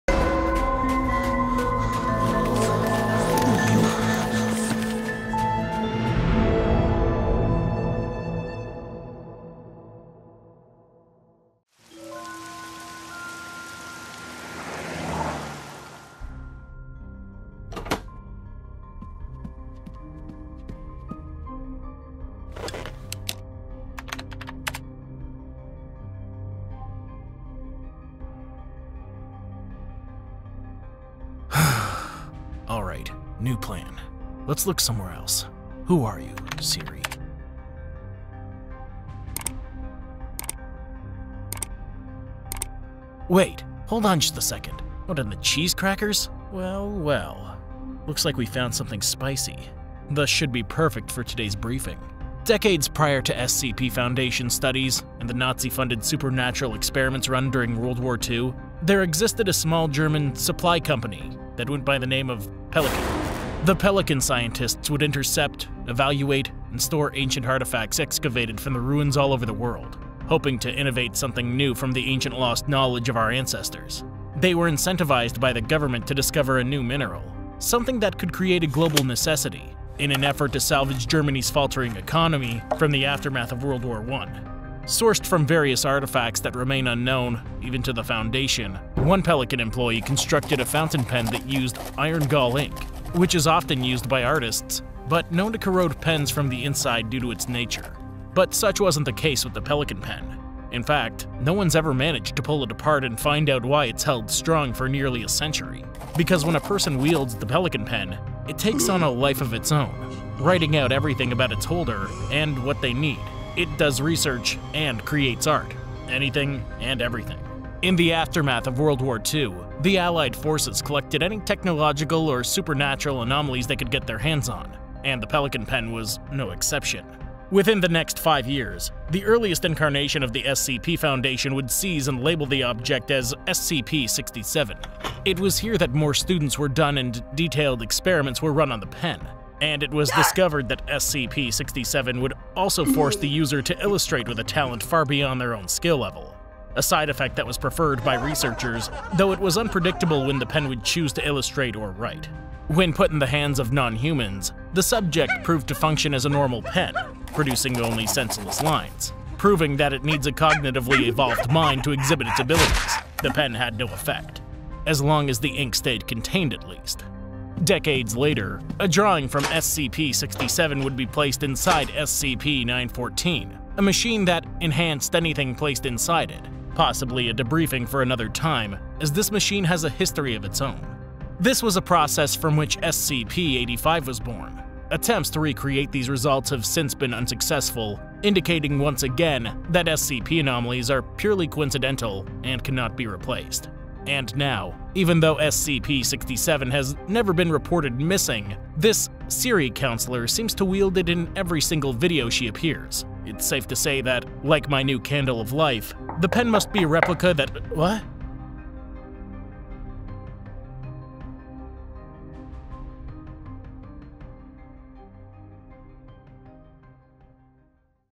Project Flare Ep.2: SCP-067 'The Artist's Pen' | Detective Void Animation
SCP-067, known as the 'Pelican Pen,' takes control of the hand, filling the paper with enigmatic biographies. These peculiar inked writings deserve attention, as they transport us into the realm of someone else's memories and emotions.

Before each release of a new video, I give a hint on the Instagram page, you have to guess it!

written by ..., is published under Creative Commons Sharealike 3.0.